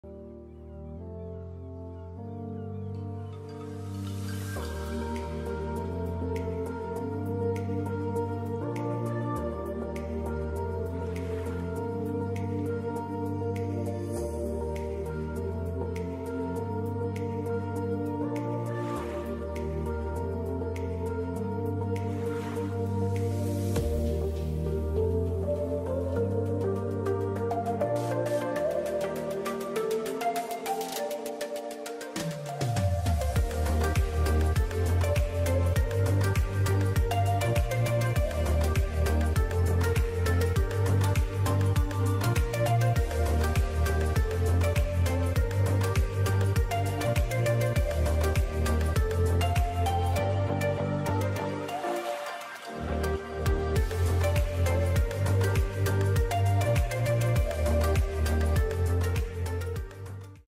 Thailand Islands - DJI Mini 3 Pro Cinematic
Aerial footage of Thailand.
All footage shots been captured by DJI Mini Pro 3.